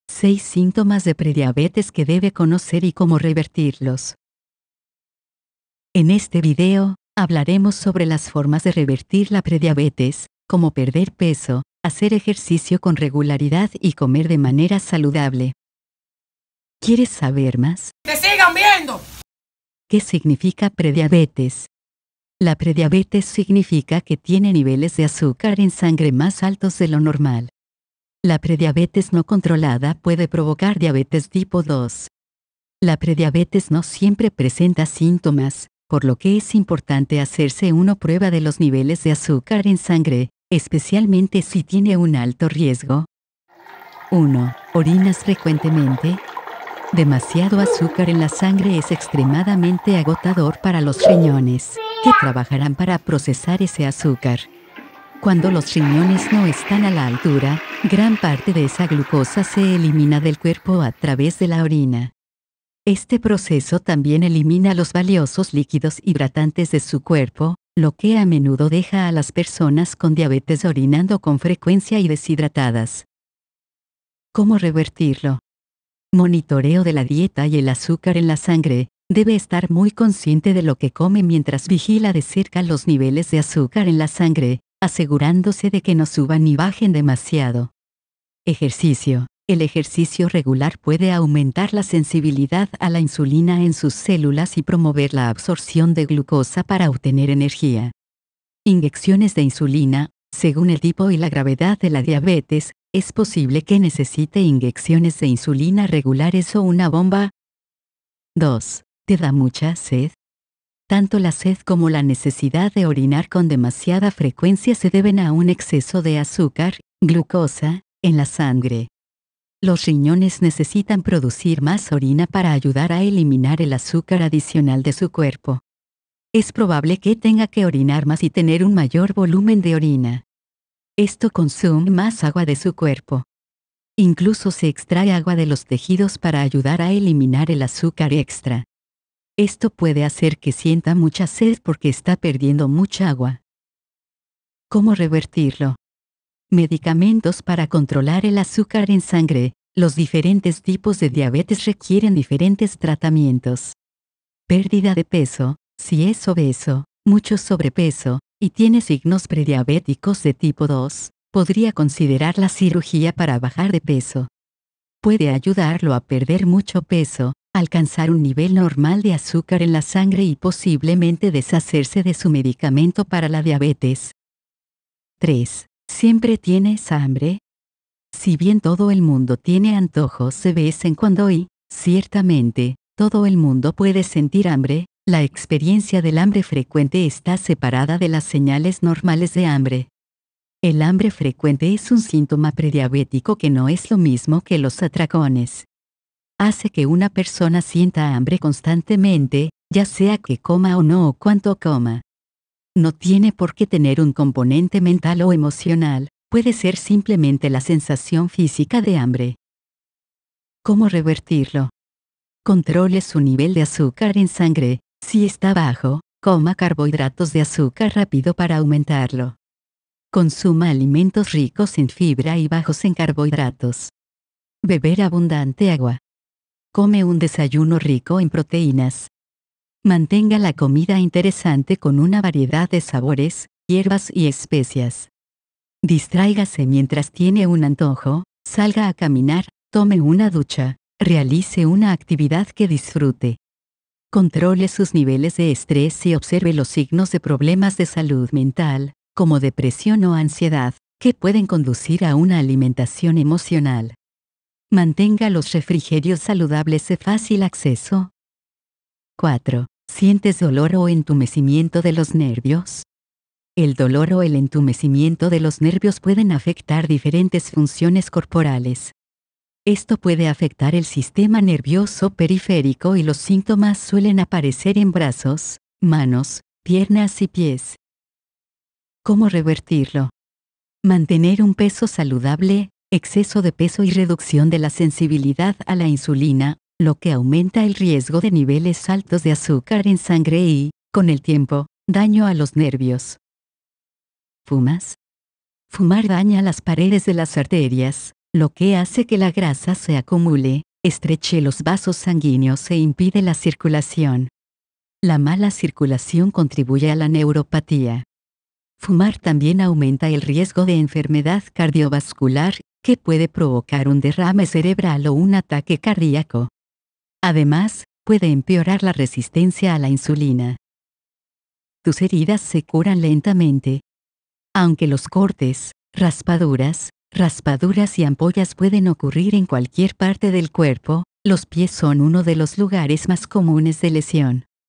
6 síntomas de prediabetes que debe conocer y cómo revertirlos
En este video, hablaremos sobre las formas de revertir la prediabetes, como perder peso, hacer ejercicio con regularidad y comer de manera saludable.

¿Quieres saber más? ¡Sigue mirando!
¿Qué significa prediabetes?
La prediabetes significa que tiene niveles de azúcar en sangre más altos de lo normal.
La prediabetes no controlada puede provocar diabetes tipo 2.
La prediabetes no siempre presenta síntomas, por lo que es importante hacerse una prueba de los niveles de azúcar en sangre, especialmente si tiene un alto riesgo.